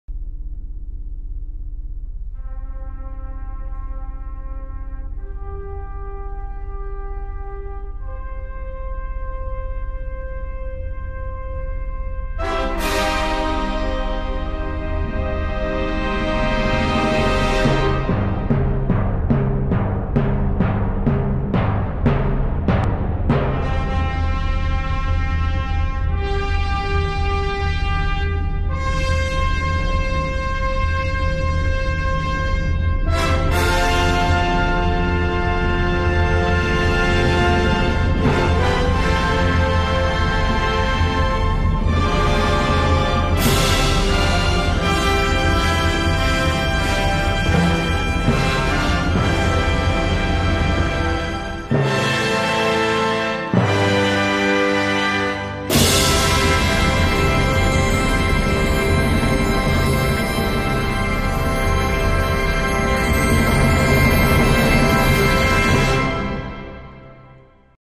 The missing punctuation...
Why is the apostrophe missing from this brand? Well, now I've done gone and fixed it.